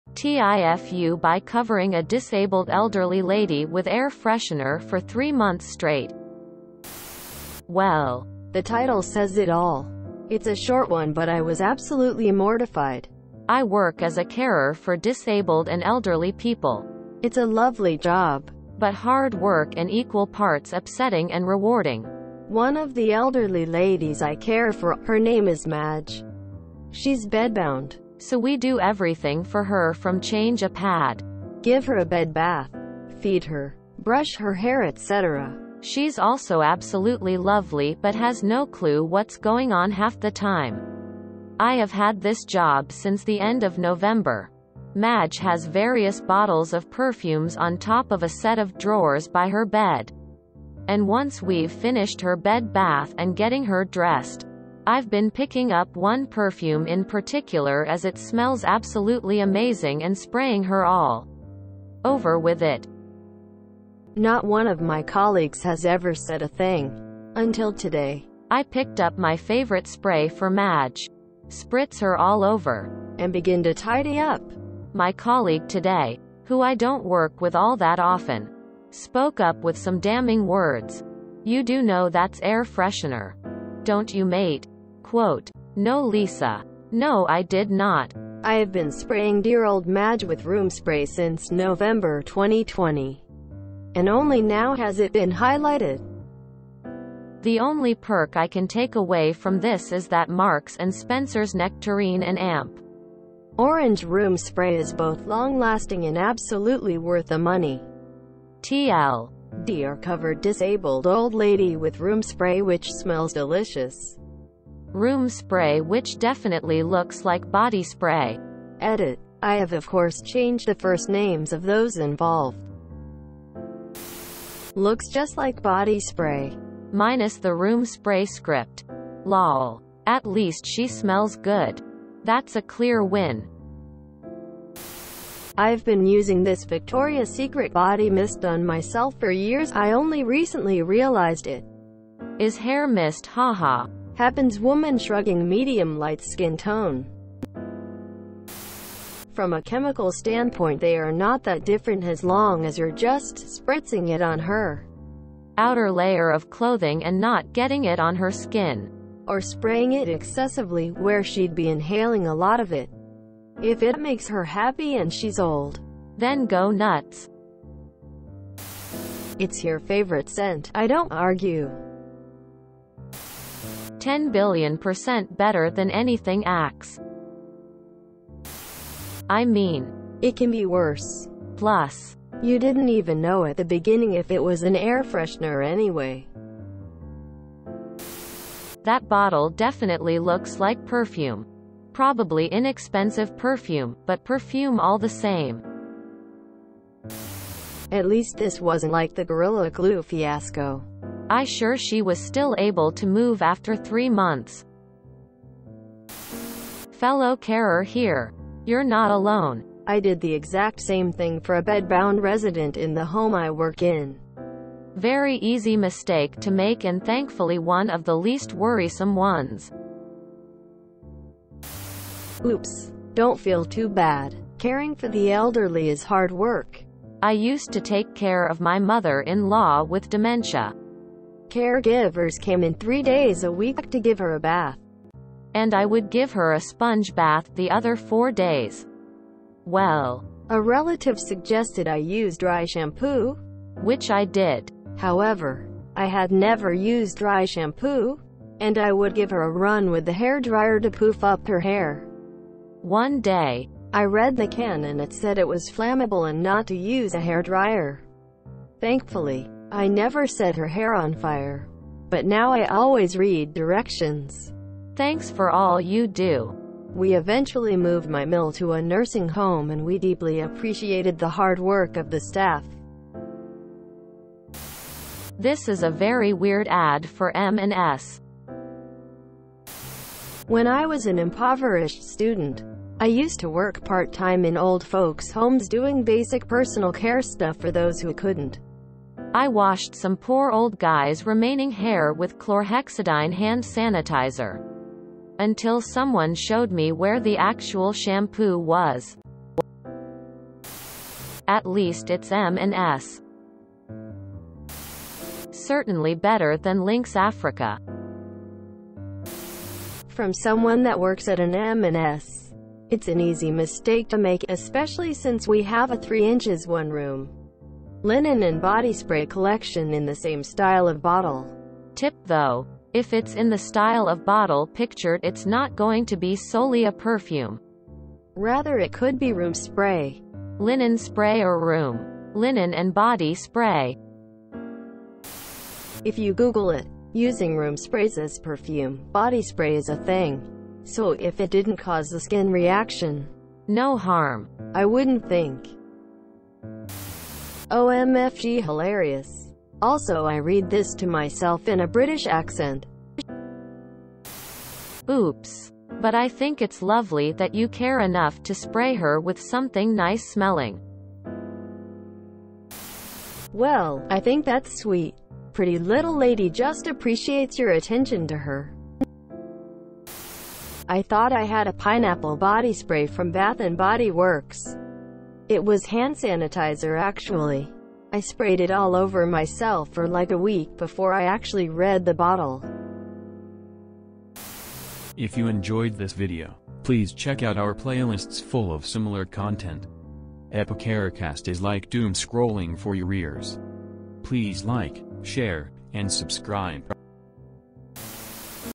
TIFU by covering a disabled elderly lady with air freshener for 3 months straight
TIFU by covering a disabled elderly lady with air freshener for 3 months straight (r/tifu)